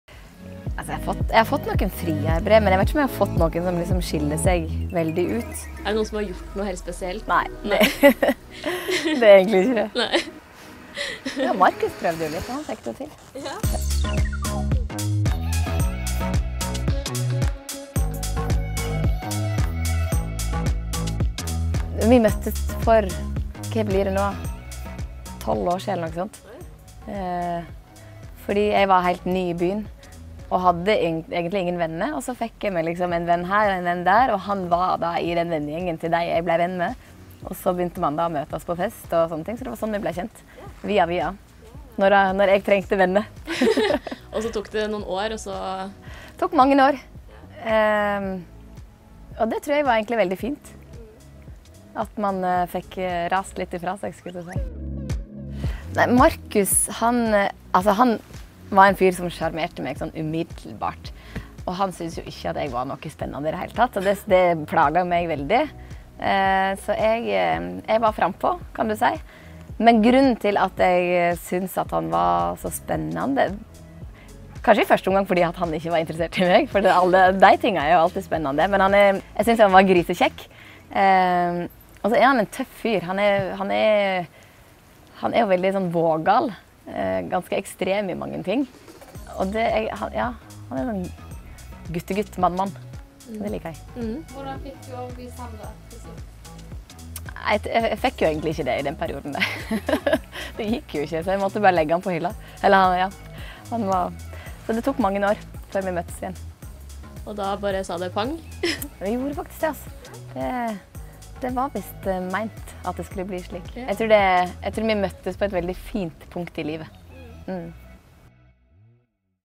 Slik falt Tone for kjæresten: - Jeg var mer interessert enn han var
KJENTE HVERANDRE LENGE: Tone Damli er aktuell som programleder for «Love Island», og i den anledning forteller hun om hvordan hun falt for kjæresten Markus Foss. De to kjente hverandre i mange år før de ble sammen. Video: Marie Røssland